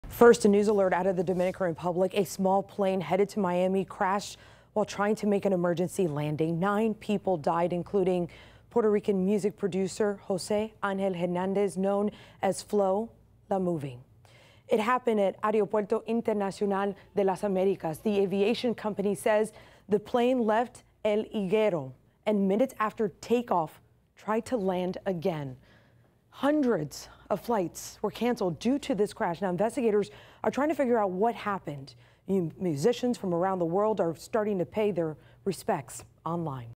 Puerto Rican music producer, Flow La Movie, among 9 dead in Florida-bound plane crash
A small plane heading to Orlando carrying nine people, including Puerto Rican music producer José Ángel Hernández, crashed Wednesday while trying to make an emergency landing in the Dominican Republic, killing everyone on board.

Helidosa Aviation Group said on Facebook that the aircraft - model HI150 Gulfstream - carried two captains and seven passengers.